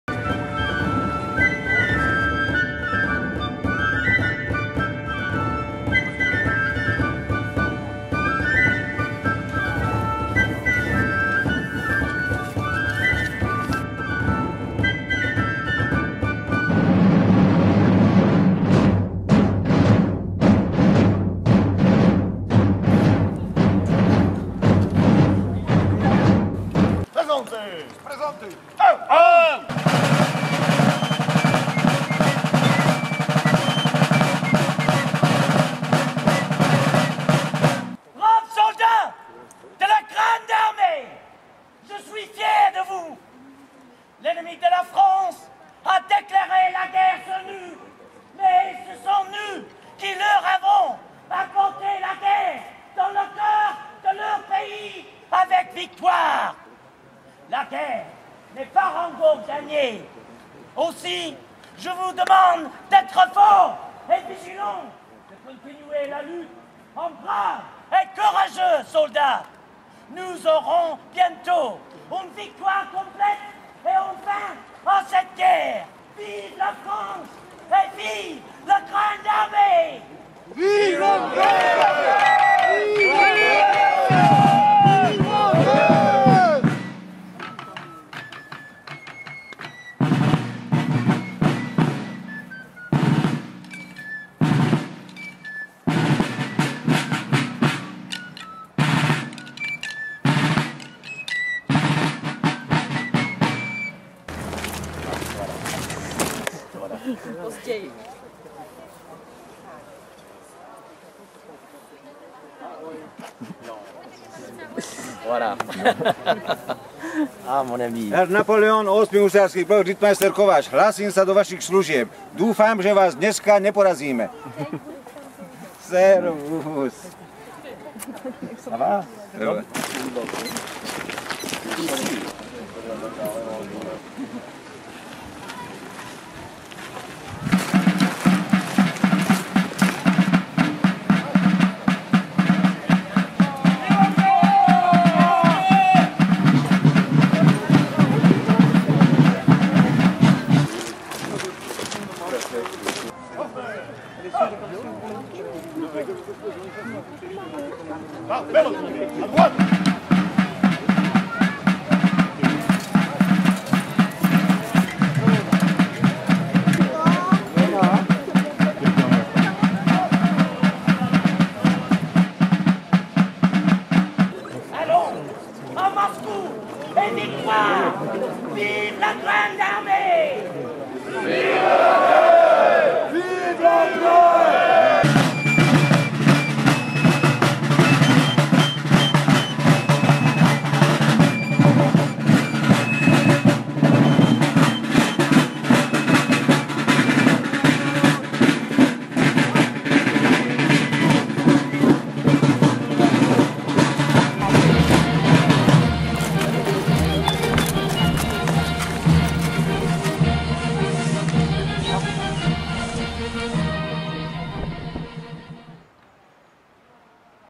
Napoleon at Austerlitz, Prepares for Battle! Napoleonské dny 2012
Napoleonic Days 2012 at Austerlitz, preparation for war!  Napoleon Bonaparte. Journées Napoléoniennes, Napoleonské dny